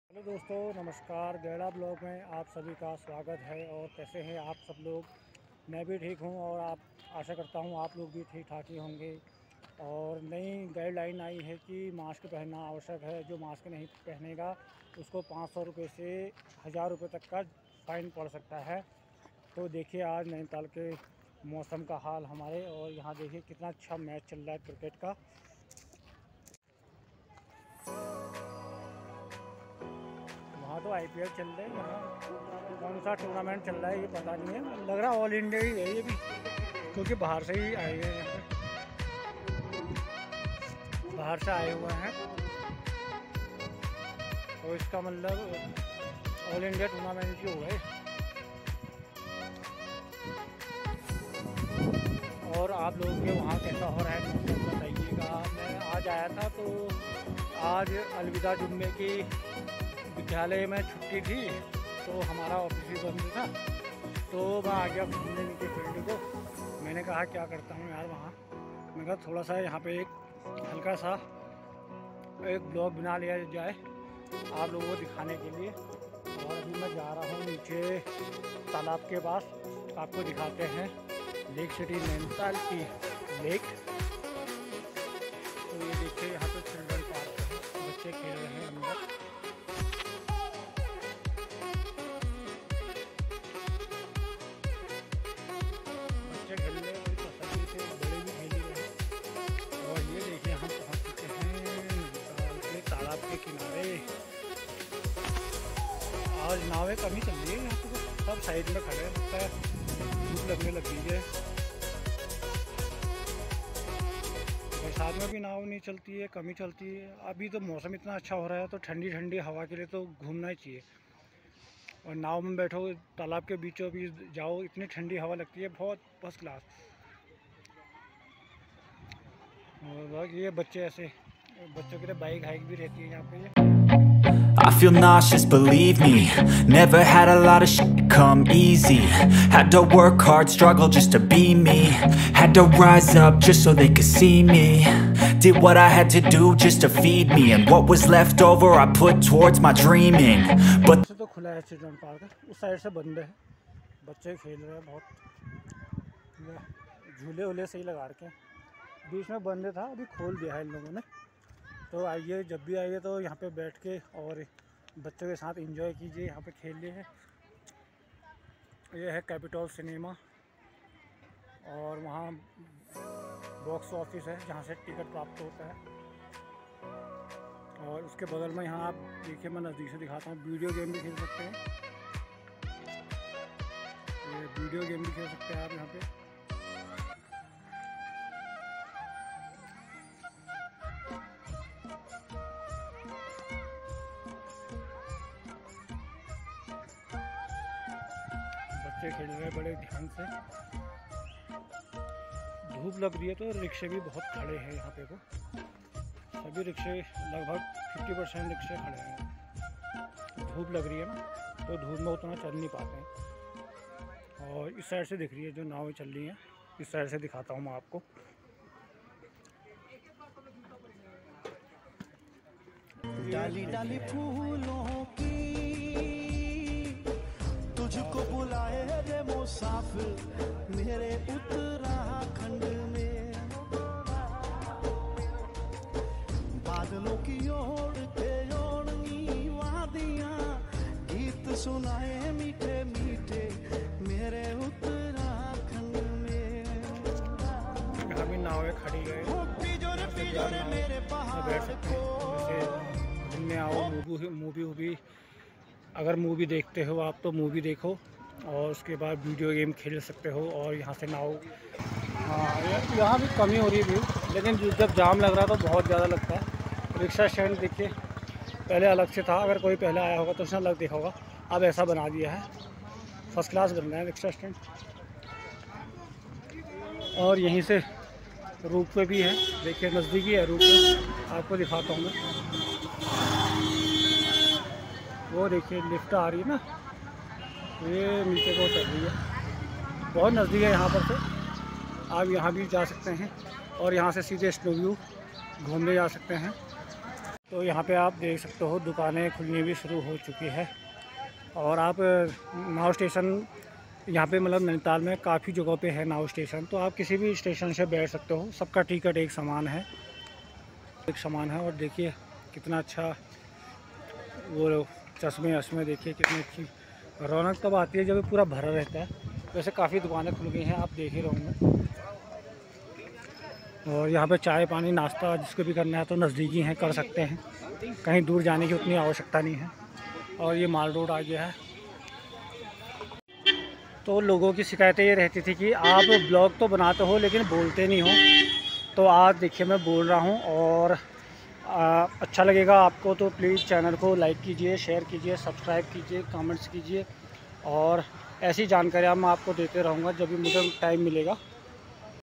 नैनीताल का मौसम और नैनीताल की जानकारी@gairavlogs-y2g educational
नैनीताल का मौसम और नैनीताल की जानकारी अगर कोई घूमने आ रहा है अभी तो वह आ सकता है क्योंकि मौसम बहुत ही सुहाना हो रहा है पर्यटक आ सकते हैं नैनीताल आइए और अपने होटल की बुकिंग करके ही आइएगा क्योंकि बाईपास से आने नहीं दे रहे हैं बाहर के गाड़ियों को बिना होटल बुकिंग और वीडियो को खूब इंजॉय करें धन्यवाद l

follow me in Istagaram ganesh gaira

follow me in Facebook ganesh gaira

this video is for only education purpose instruction for Lake City Nainital weather boating cricket match and some activities thanks

Channel name Gaira vlogs